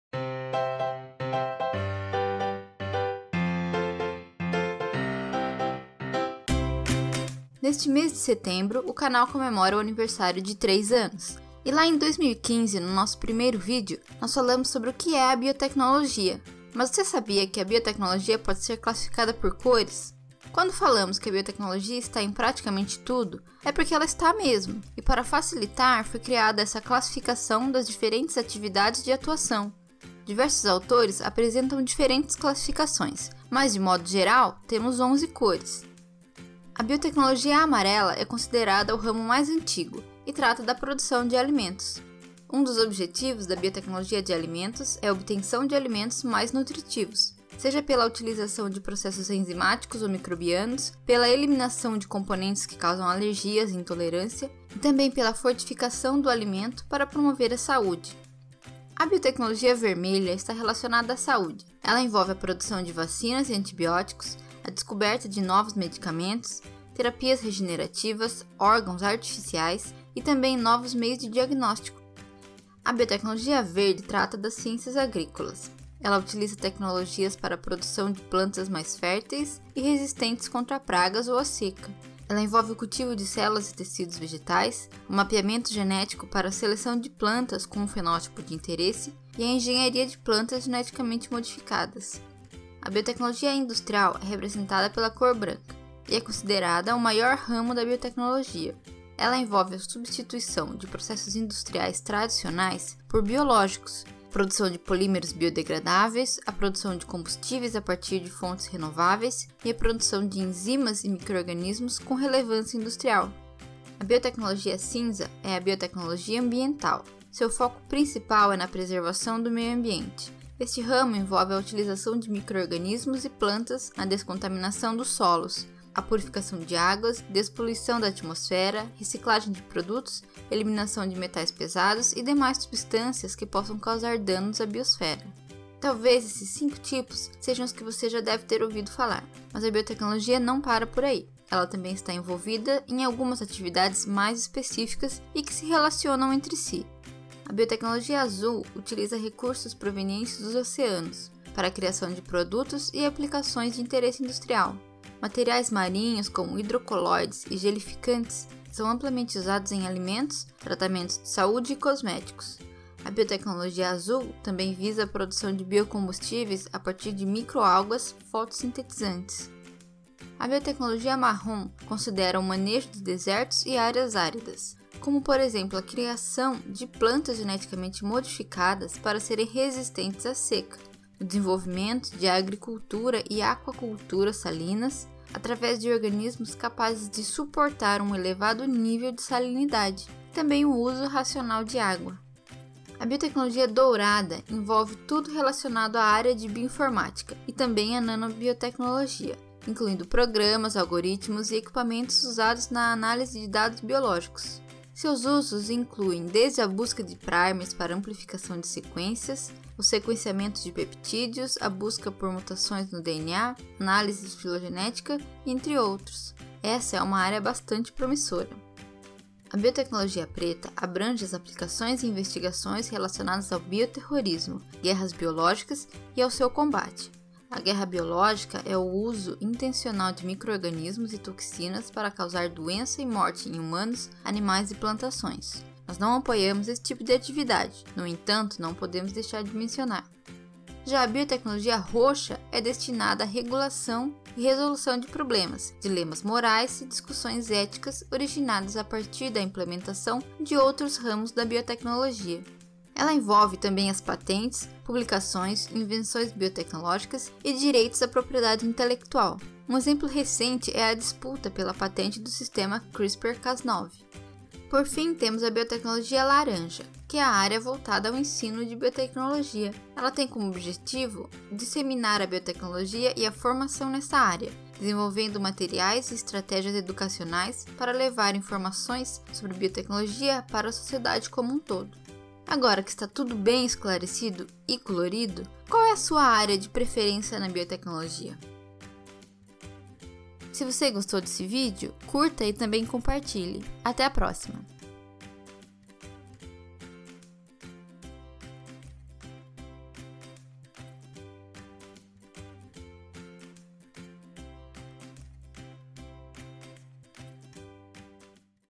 Quais as áreas de atuação da biotecnologia? InstanteBiotec 70
Neste mês de setembro o canal comemora o aniversário de 3 anos. E lá em 2015, no nosso primeiro vídeo, nós falamos sobre o que é a biotecnologia. Mas, você sabia que a biotecnologia pode ser classificada por cores? Conheça as diversas áreas de atuação da biotecnologia.

Roteiro e apresentação: Bárbara
Pesquisa e apoio: Pablo

Referências:
Biotecnología Sí. Los colores de la biotecnología.

DaSilva EJ. The Colours of Biotechnology: Science, Development and Humankind. Eletronic Journal of Biotechnology. 2004; 7(3).

Kafarski P. Rainbow code of biotechnology. CHEMIK. 2012; 66(8):811-6.

García-Escudero R. Biotechnology’s eleven colours. Mariscal Abogados.

Wikipedia contributors. Colors of biotechnology. Wikipedia, The Free Encyclopedia. 2018.